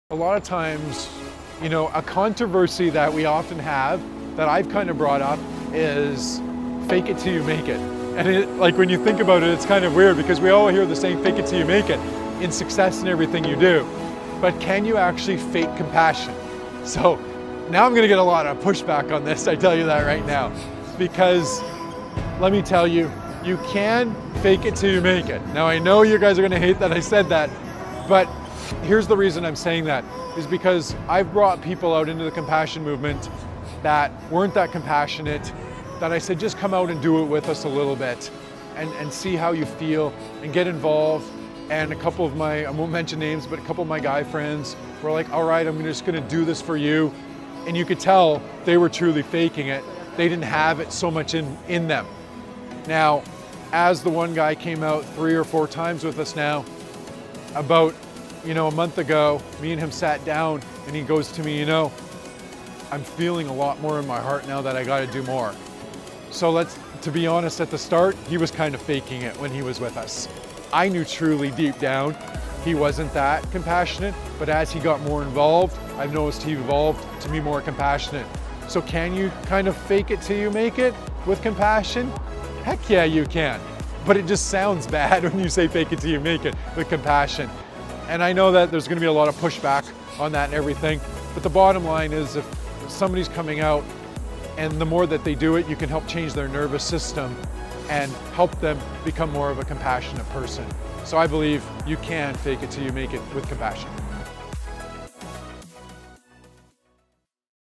Can You Fake Compassion | Shane Jeremy James
Shane Jeremy James talks about can you fake compassion.

What do you think? Should people fake empathy and compassion. Do you consider these skills to be learned in order for people to succeed in today's workplace?

Can You Fake Compassion?